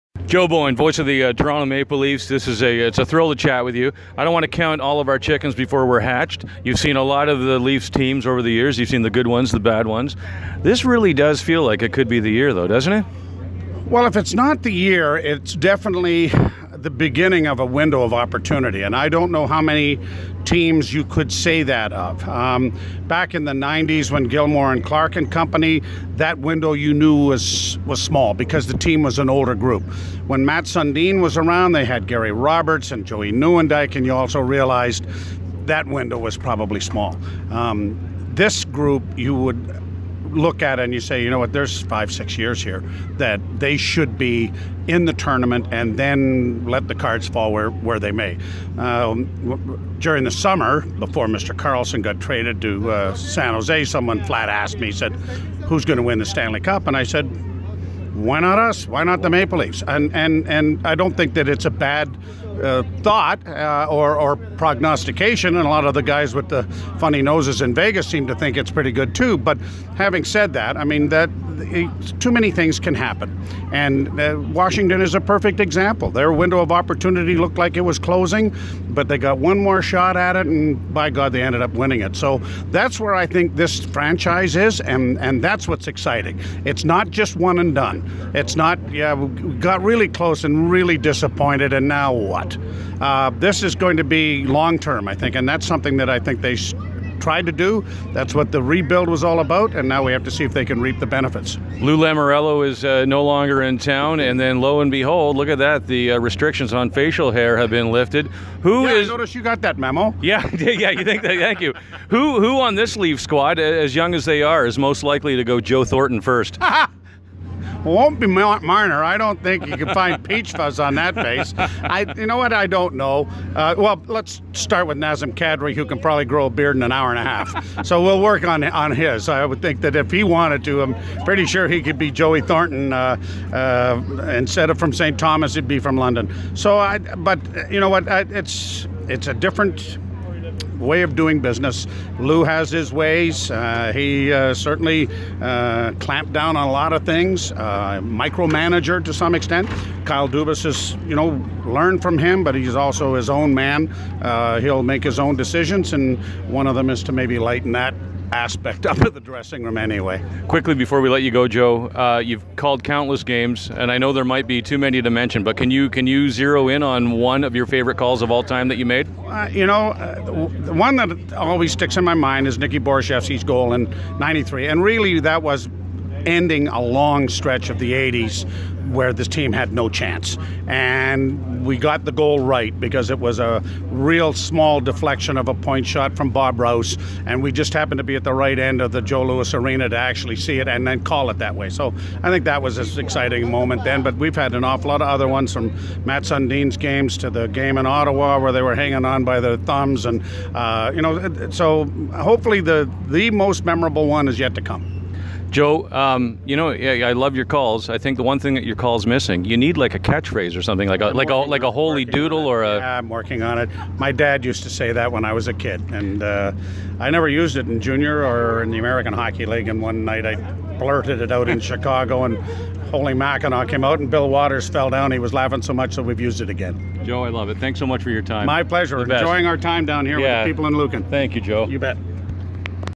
Steve Stax Chats With Leafs Broadcast Legend Joe Bowen
During Kraft Hockeyville in Lucan, Stax caught up with Joe Bowen, one of his broadcasting heroes, and asked him about Lord Stanley being hoisted by the Blue in White in 2019, facial hair freedom, and his most memorable call of all time.